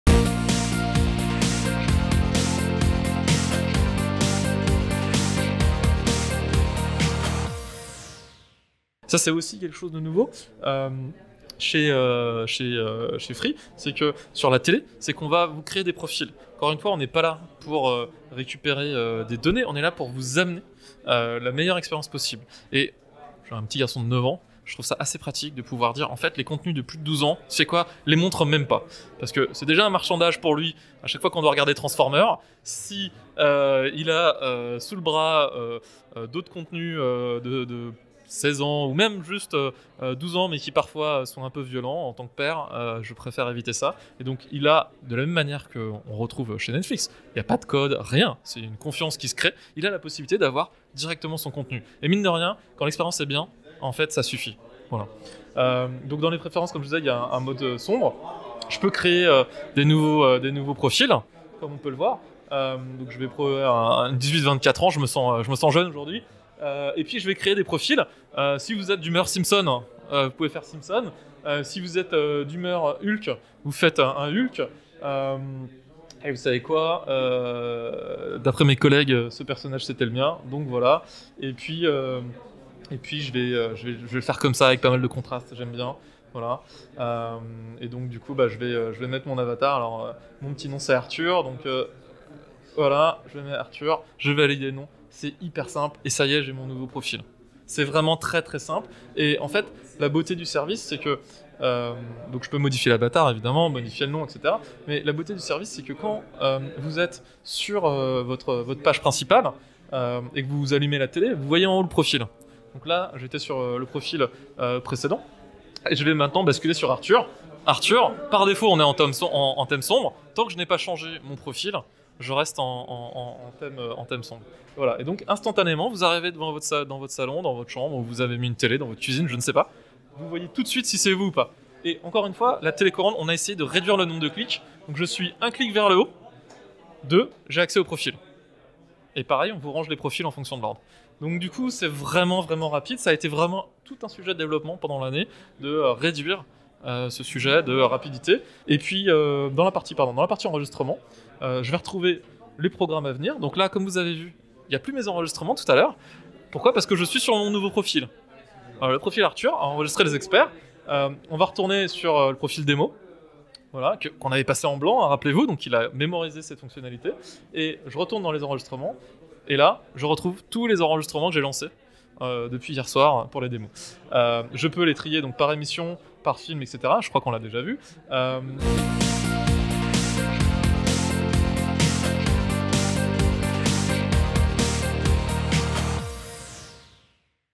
Freebox Pop : Découvrez la création de profils et d’avatars sur l’interface TV
Créez votre propre profil afin que la Freebox POP vous propose des contenus adaptés à vos goût

Suite et fin de notre balade au travers d’OQee, l’interface TV de la Freebox Pop, en compagnie d’Arthur, un des développeurs Free qui est à l’origine de sa conception. A l’image de ce qui existe sur d’autres services audiovisuels cette nouvelle Freebox Pop propose la création de différents profils, qui permettent en particulier de choisir l’age (et donc d’avoir accès aux seuls contenus autorisés) ou encore le mode clair ou sombre. Chaque profil dispose également de son propre espace "enregistrements", où seuls ses programmes enregistrés seront disponibles et se voit également recommander des contenus liés à ses goût et habitudes.

Pour chaque profil créé, il sera proposé de de concevoir avatar personnalisé. Vous pouvez ainsi créer un visage qui vous ressemble, avec les couleurs qui vous plaisent. Pour passer d’un profil à l’autre, rien de plus simple, tous sont accessibles en permanence en haut à droite dans l’interface OQee, il suffit de cliquer dessus pour passer de l’un à l’autre, sans qu’aucun code ne soit demandé.